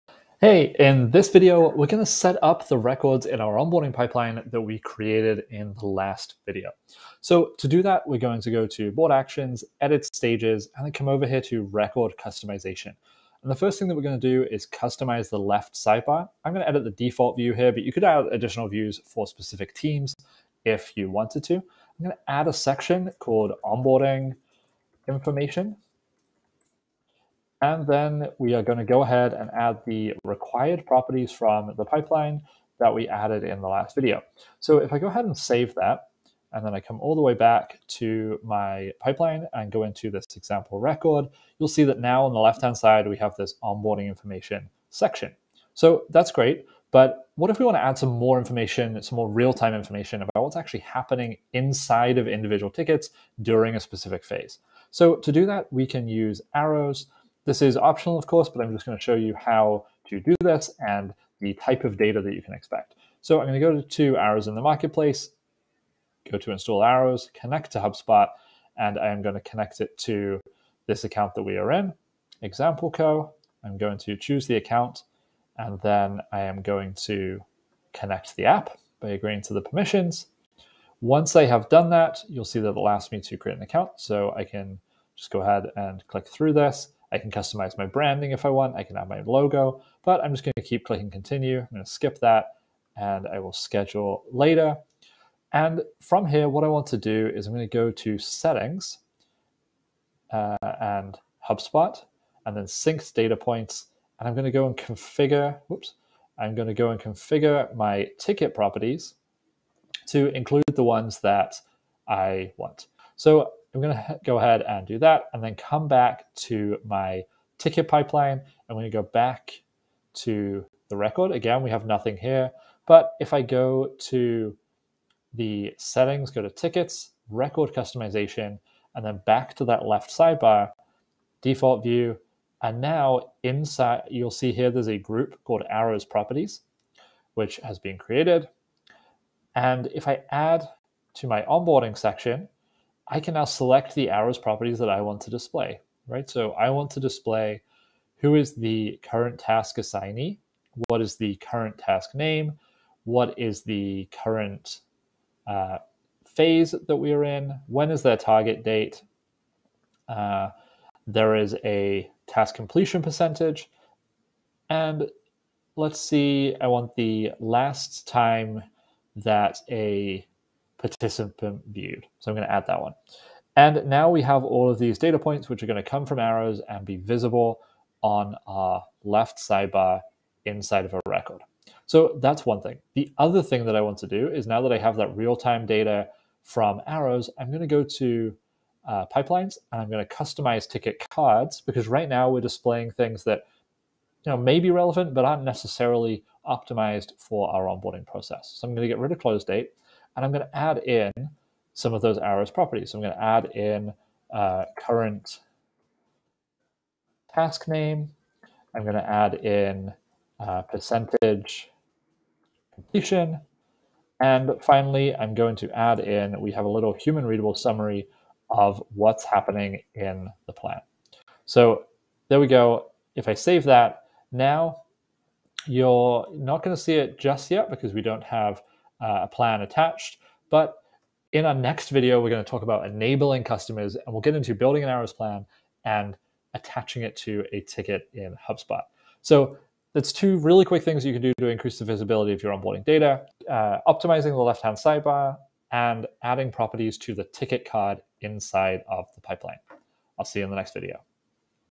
HubSpot Ticket Setup For Customer Onboarding (No More Spreadsheets)
Running customer onboarding in HubSpot instead of a spreadsheet means much less manual updating of data and the possibility of integrating real-time data from tools like Arrows. But not every team wants to see the same thing in their onboarding records so HubSpot allows us to customize the experience for each team to give them the information they need when they need it and that's what we'll cover in this video.

💘WHAT IS ARROWS?
Arrows is a collaborative customer onboarding tool that connects to HubSpot, for teams using HubSpot pipelines to manage their customer onboarding process.

⏰ TIMESTAMPS
00:00 Customizing the left sidebar in HubSpot records
00:48 Adding real-time onboarding data with Arrows
02:00 Adding Arrows data to the left sidebar in onboarding records
03:03 Adding required fields to your HubSpot pipeline